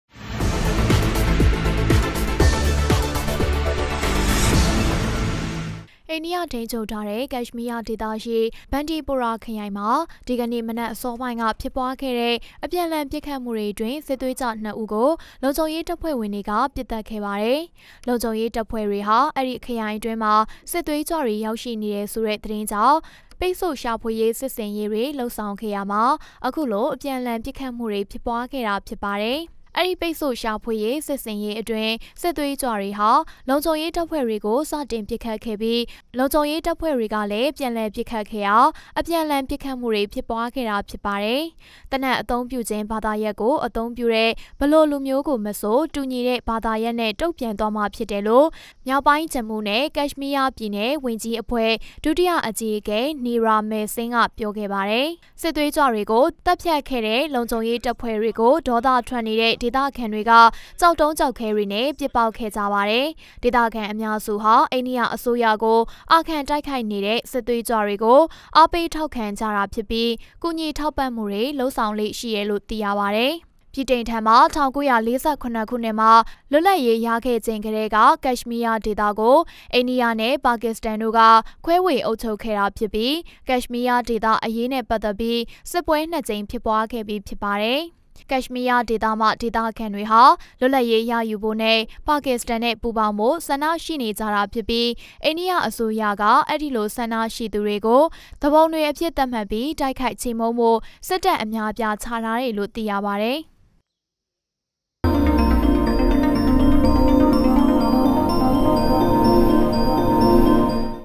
အိႏၵိယထိန္းခ်ဳပ္ထားသည့္ ကတ္ရွ္မီးယားေဒသတြင္ အျပန္အလွန္ပစ္ခတ္မႈအတြင္း...
အိႏၵိယထိန္းခ်ဳပ္ထားတဲ့ ကတ္ရွ္မီးယားေဒသရွိဘန္ဒီပိုရာခရိုင္တြင္ ဒီကေန႕ မနက္အေစာပိုင္း က ျဖစ္ပြားခဲ့တဲ့ အျပန္အလွန္ပစ္ခတ္မႈေတြအတြင္းစစ္ေသြးၾကြ ၂ ဦးကိုလံုၿခဳံေရးတပ္ဖြဲ႔ေတြကပစ္သတ္ခဲ့ပါတယ္။